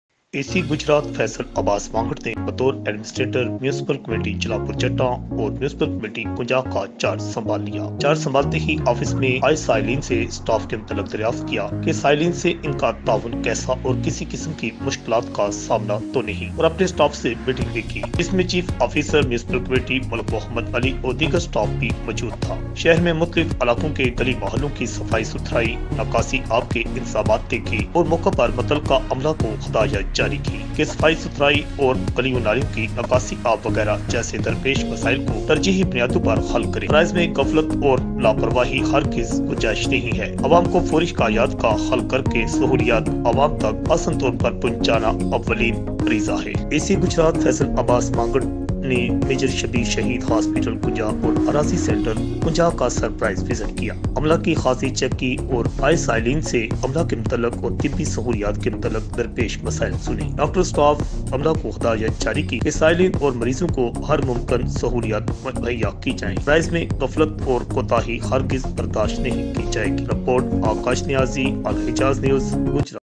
Ac gujrat faisal Mangat ko mc jalalpur jattan air kunjah ka charge b mil geya achanik chapay cheking
اے سی گجرات فیصل عباس مانگٹ کو میونسپل کمیٹی جلالپورجٹاں اور کنجاہ کا بھی اضافی چارج دے دیا گیا نئے عہدوں کا چارج سنبھالتے ھی اے سی گجرات فیصل عباس مانگٹ نے کیا کیا دیکھئے الحجاز نیوز کی اس رپورٹ میں